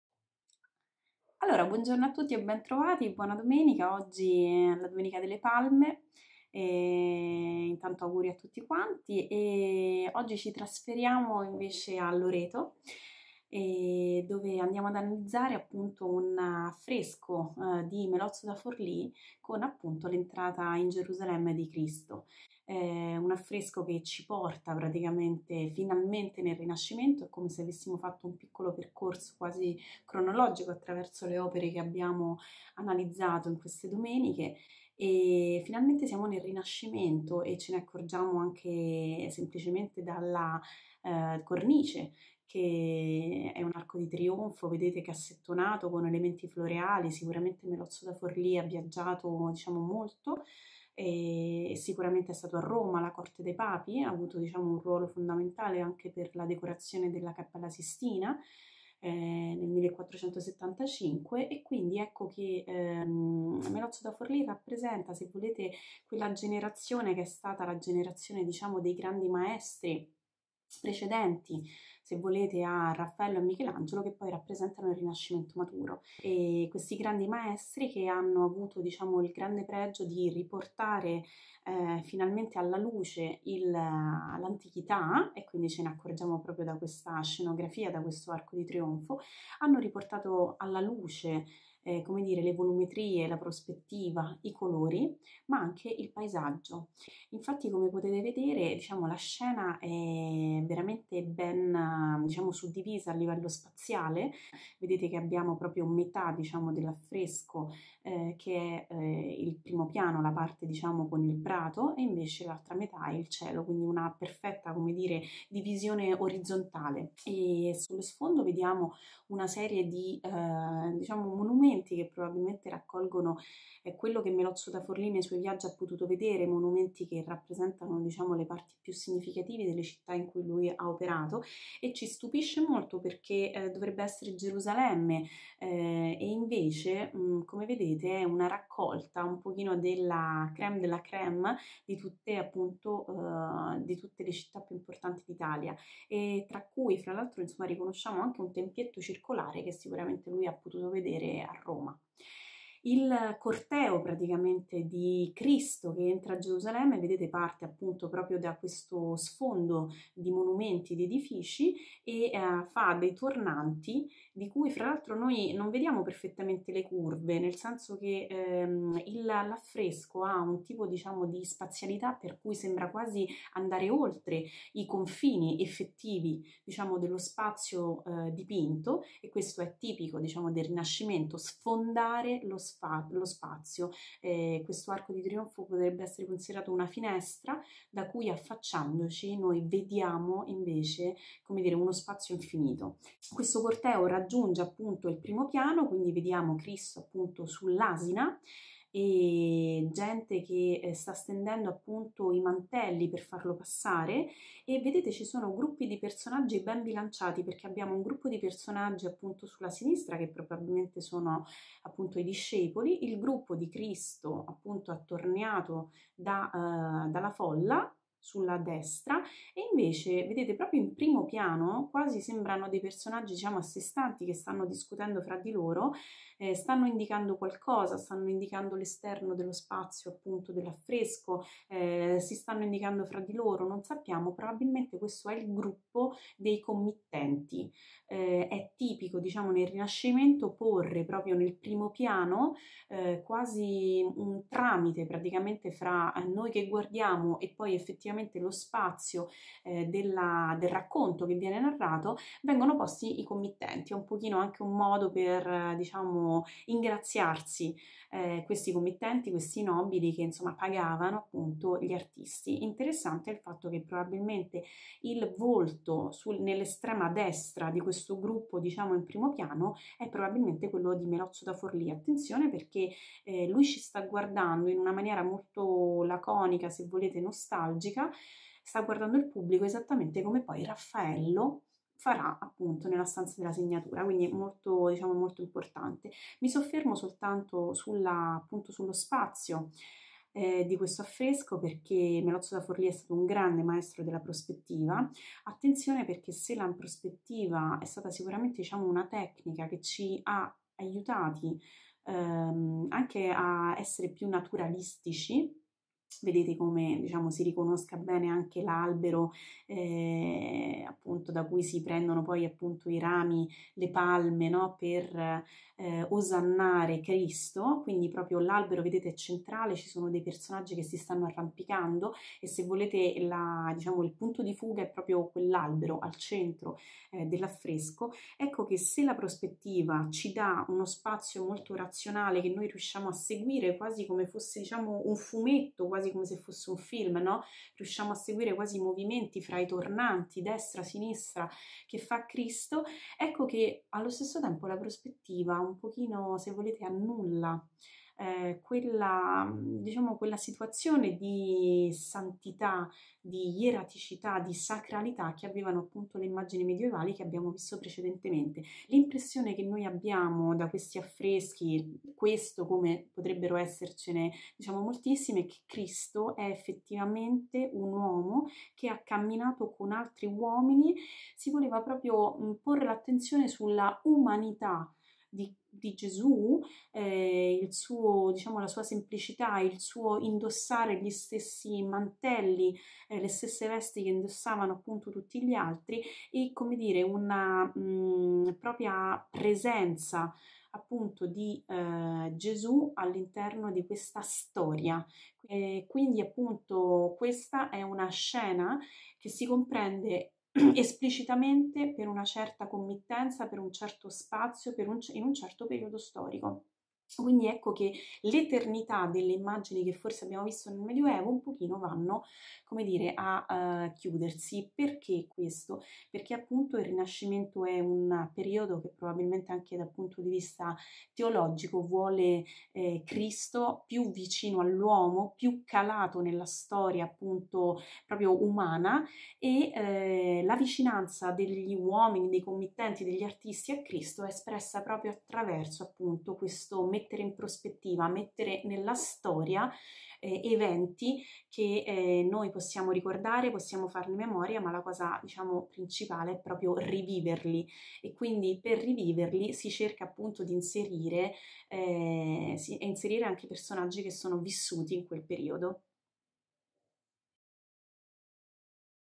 Ingresso di Gesù a Gerusalemme di Melozzo da Forlì
Continua il viaggio del Settore Adulti di Ac di Roma attraverso gli esercizi di fede e cultura che accompagnano il Vangelo della domenica. L'opera di questa puntata è l'ingresso di Gesù a Gerusalemme di Melozzo da Forlì. L'affresco fa parte della Sagrestia di San Marco che è una delle quattro sagrestie del santuario della Santa Casa di Loreto come ci spiega Monica Panizzoli nel suo video di oggi.
Si ringrazia Fabio Spinelli per la parte tecnica.